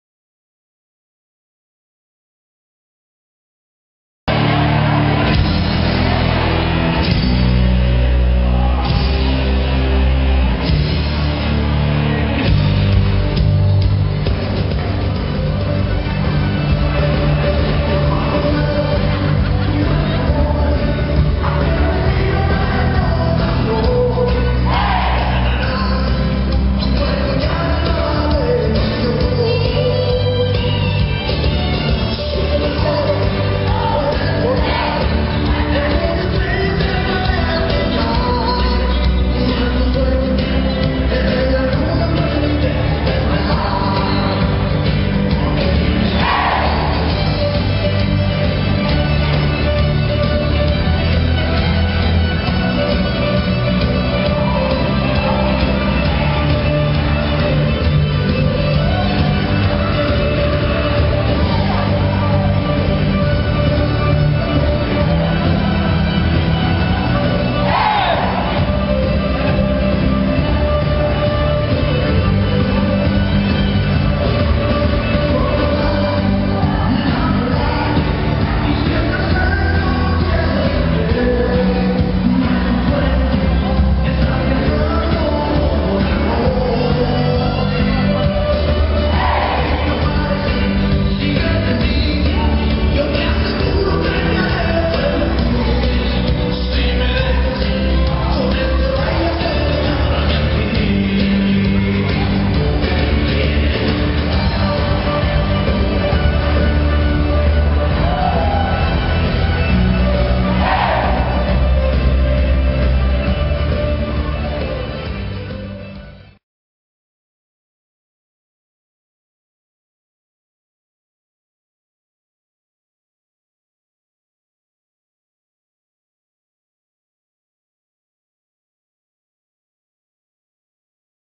2ª TROBADA DE LES TERRES DE L'EBRE -TEXAS VALEY-.
2ª TROBADA DE LES TERRES DE L'EBRE  TEXAS VALEY -
GRUPS ,  ENGANXATS AL COUNTRY&LINE DANCE, LES SHERIFFES, COR COUNTRY, PREMIA AVECA.
(camara Asensi)